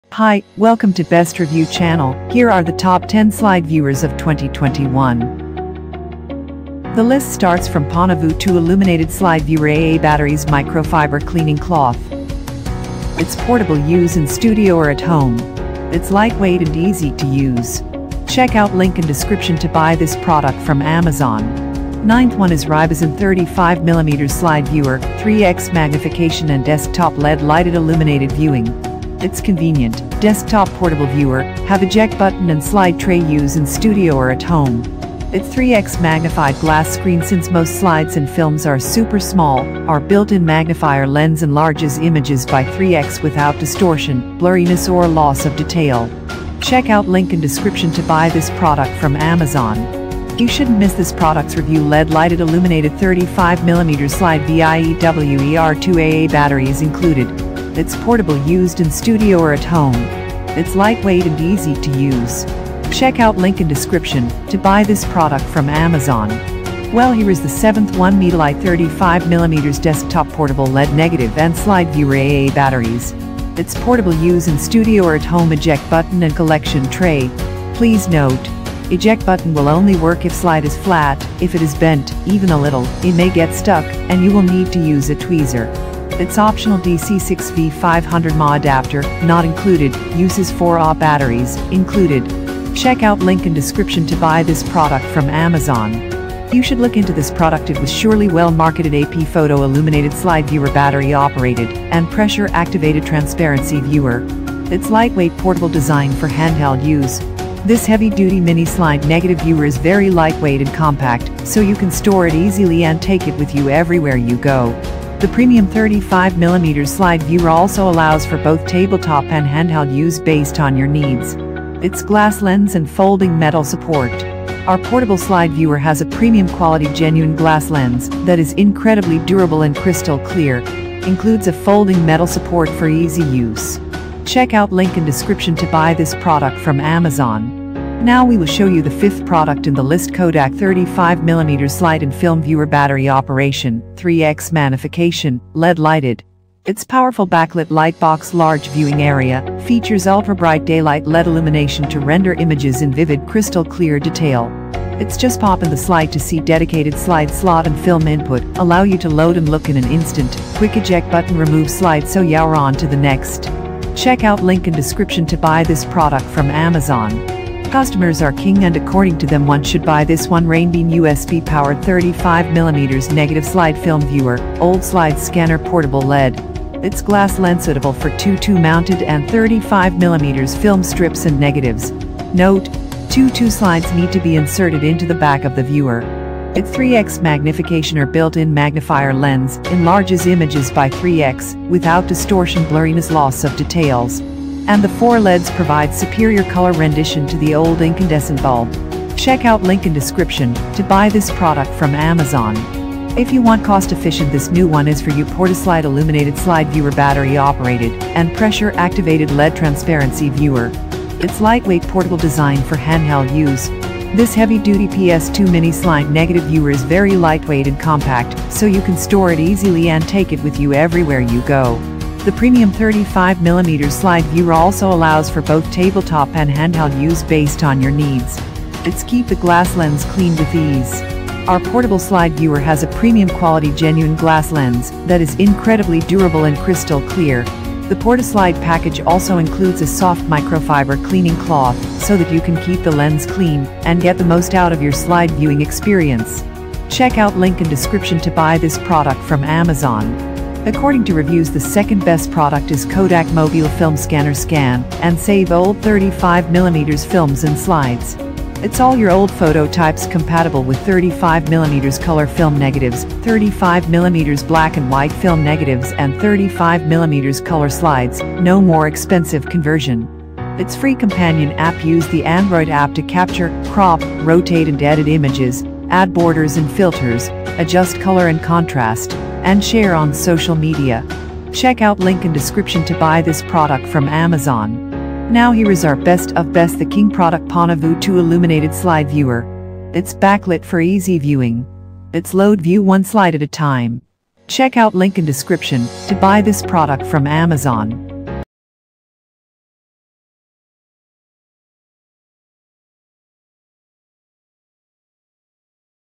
👉 Top 10 Slide Viewers 2021 (Review Guide)
👉 1
Pana-Vue 2 Illuminated Slide Viewer
👉 2
KODAK Mobile Film Scanner – Scan & Save Old 35mm Films & Slides
👉 3
Porta Slide Illuminated Slide Viewer Battery Operated & Pressure Activated LED Transparency Viewer
👉 4
RAINBEAN USB Powered 35mm Negative Slide Film Viewer, Old Slides Scanner Portable LED
👉 5
KODAK 35mm Slide and Film Viewer - Battery Operation, 3X Magnification, LED Lighted
👉 6
AP Photo Illuminated Slide Viewer Battery Operated & Pressure Activated Transparency Viewer
👉 7
Medalight 35mm Desk Top Portable LED Negative and Slide Viewer + AA Batteries
👉 8
LED Lighted Illuminated 35mm Slide Viewer(2AA Batteries Included)
👉 9
Rybozen 35mm Slide Viewer, 3X Magnification and Desk Top LED Lighted Illuminated Viewing
👉 10
Pana-Vue 2 Illuminated Slide Viewer + AA Batteries + Microfiber Cleaning Cloth

After 100's of customers and editors reviews of Top 10 Slide Viewers, we have finalised the above mentioned Best 10 products.